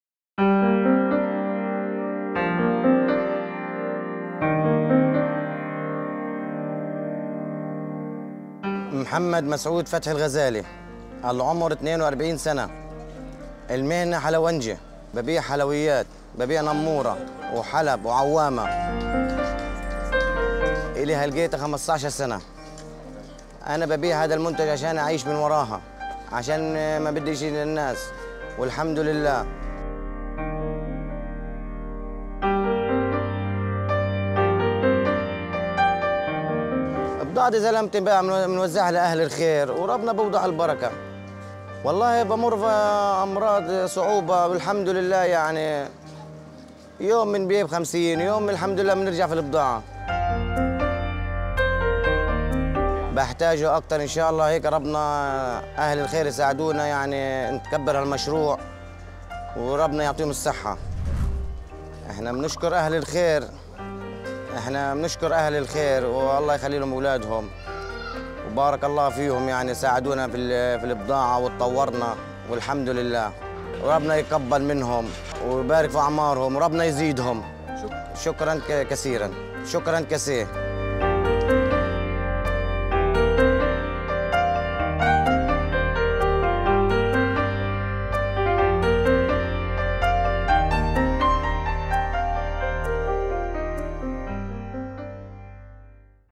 Pedagang Gaza: “Kalau Gak Habis, Saya Bagi-bagi Aja”
“Kalau gak habis dagangannya, saya bagi-bagi”
Masya Allah, mulia sekali hati pedagang Gaza ini.

Gak takut untuk rugi saat dagangannya gak habis, ia malah bagi-bagi ke orang lain. Bahkan justru membuatnya merasa berkah.

Sahabat Kasih Palestina, banyak sekali pedagang Gaza yang berpotensi untuk kita berdayakan ekonominya. Mau bantu borong dagangan mereka gak?